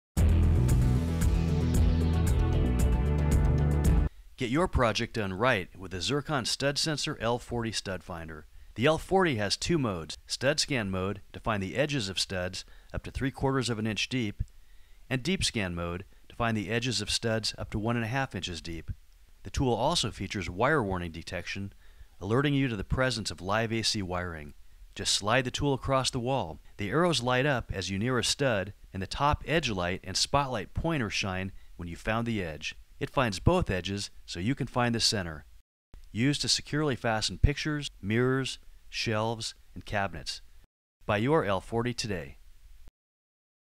Zircon StudSensor L40 Edge Finding Stud Finder
Zircon StudSensor L40 edge finding stud finder with DeepScan® mode quickly and easily finds the edges of wood and metal studs and joists up to 1 1/2 inches deep. Its easy-to-read LED display indicates stud edges, the patented SpotLite® Pointer shines a beam of light on the wall to identify the target, and WireWarning® detection indicates the presence of live AC voltage. Its patented pinch grip and contoured shape provide an easy, secure hold in either hand, at any angle.
* Not recommended for lath & plaster walls *

* A BRAND NEW 9V ALKALINE BATTERY IS REQUIRED FOR OPERATION. *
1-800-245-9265 Toll-free, Mon - Fri. 8am - 5pm PST (excluding holidays)

Caution: Do not rely exclusively on scanners to locate objects behind a scanned surface. Some objects may not be detectable by your scanner. Use additional information sources such as visible points of entry of pipes and wiring into walls, standard 16” and 24” (41 – 61 cm) stud spacing practices, and construction plans to determine the likely location of hidden objects. Always use caution before drilling or cutting.

WARNING: ALWAYS TURN OFF THE ELECTRIC, GAS, AND WATER SUPPLIES BEFORE PENETRATING A SURFACE. NEVER ASSUME THERE ARE NO LIVE ELECTRICAL WIRES IN A WALL. FAILURE TO FOLLOW THESE INSTRUCTIONS MAY RESULT IN ELECTRIC SHOCK, FIRE, SERIOUS INJURY, AND/OR PROPERTY DAMAGE.